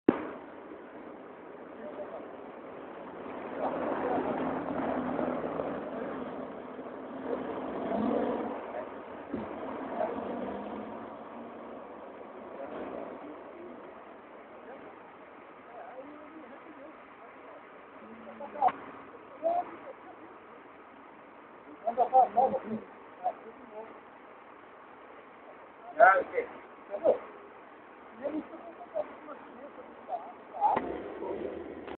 Caio Gabriela
Um antigo Caio Gabriela trafegando numa linha da cidade de Bananal-SP
Embora eu não curta andar num desses, olhando pelo lado da história
dos transportes fiz o vídeo.